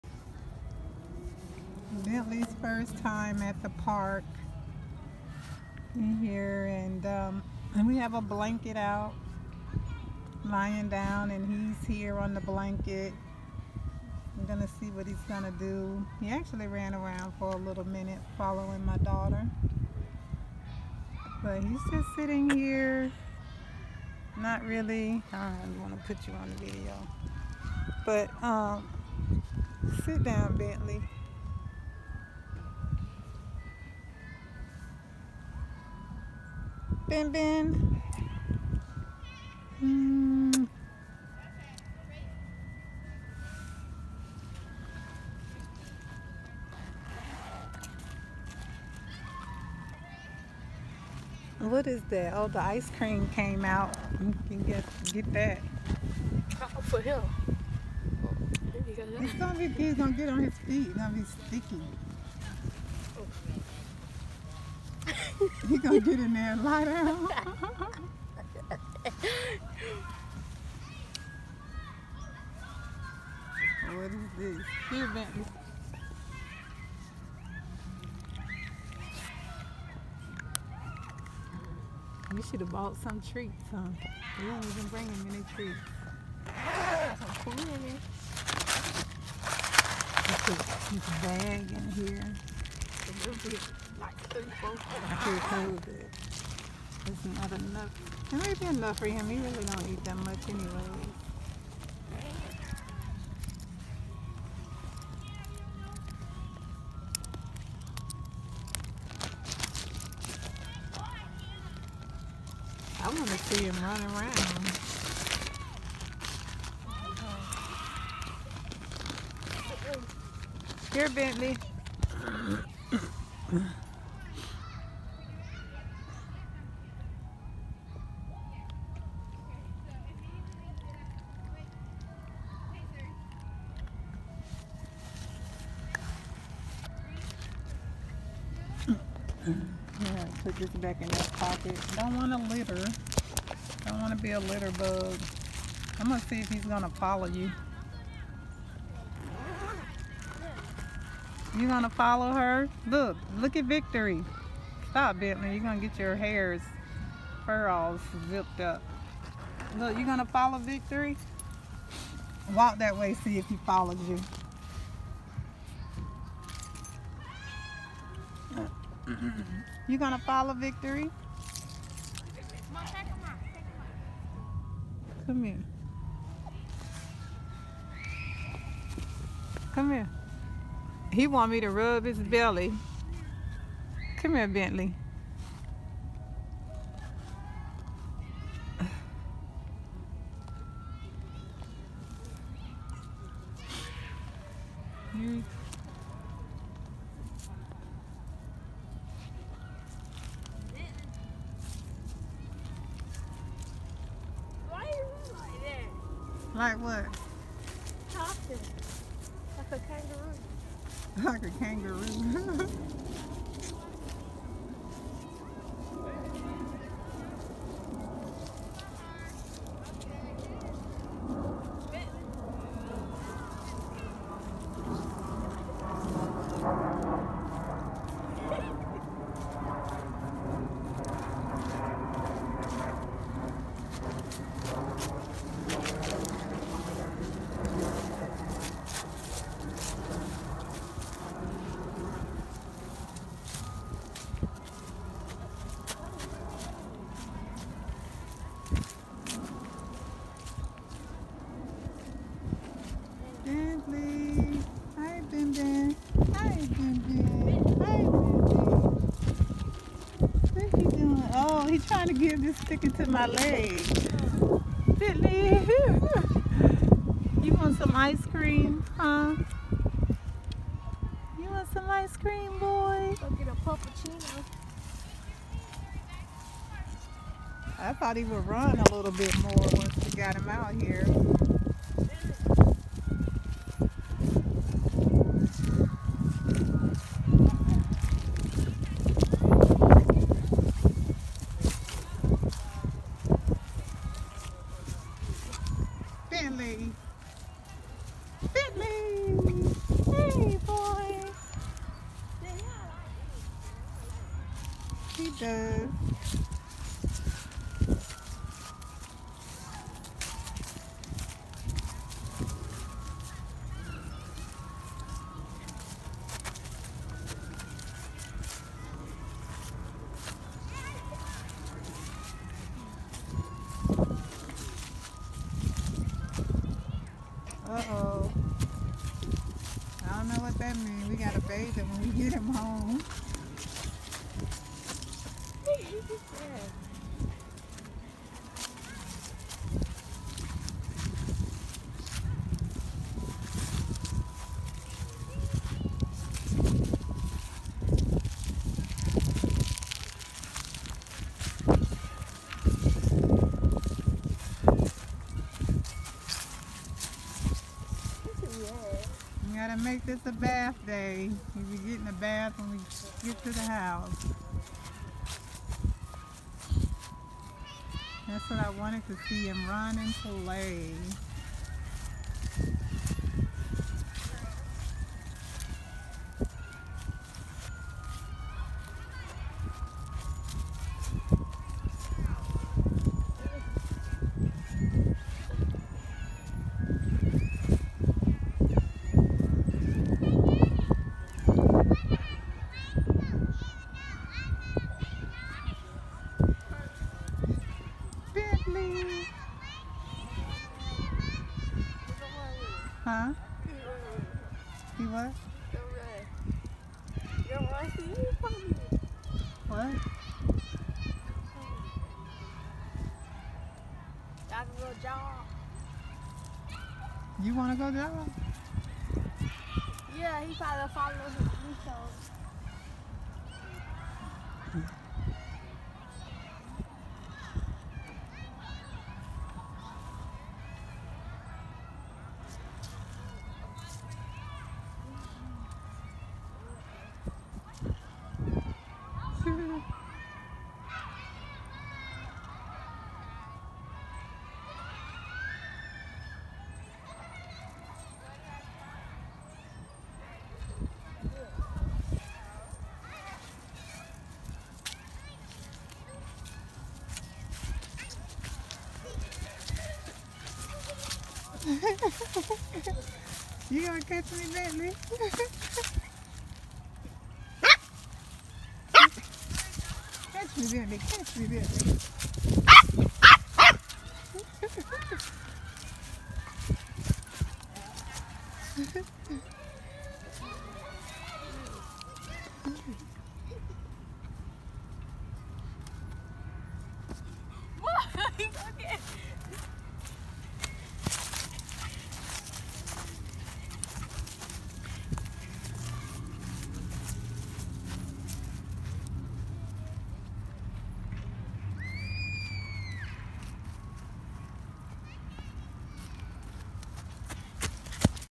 Puppy First Trip To The Park
Puppy Bentley’s First Time Playing In The Park.